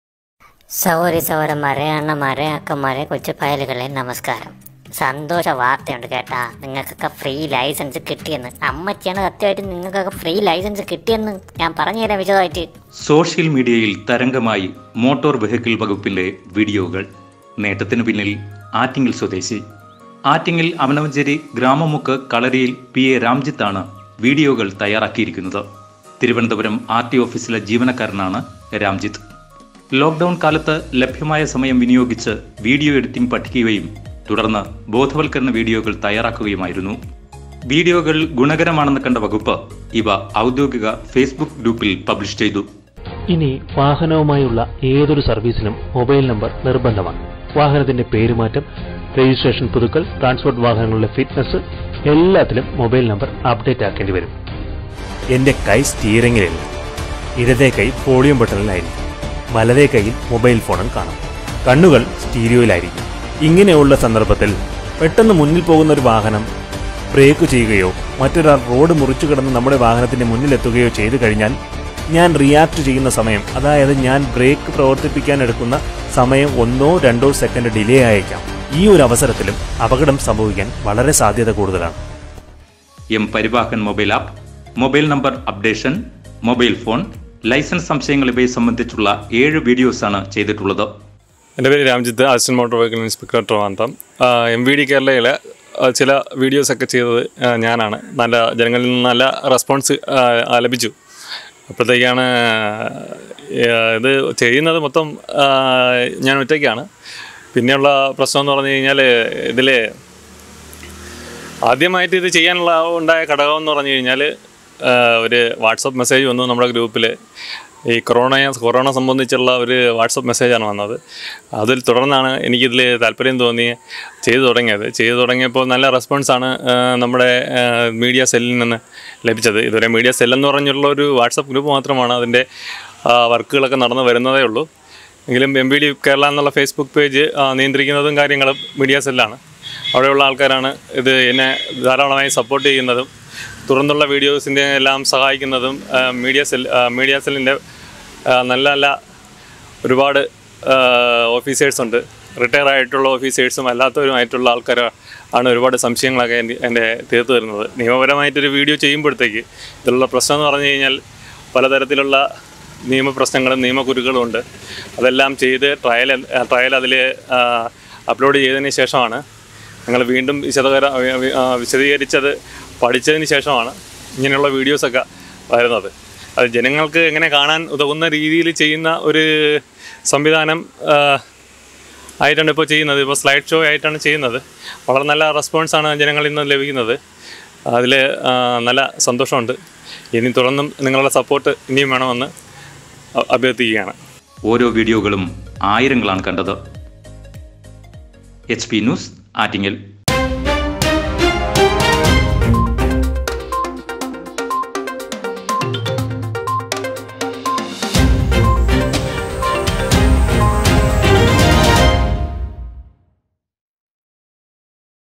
M V D
സോഷ്യൽ മീഡിയകളിൽ തരംഗമായ ഗതാഗത വകുപ്പിന്റെ ബോധവൽക്കരണ വീഡിയോകൾക്ക് പിന്നിൽ ആറ്റിങ്ങൽ സ്വദേശി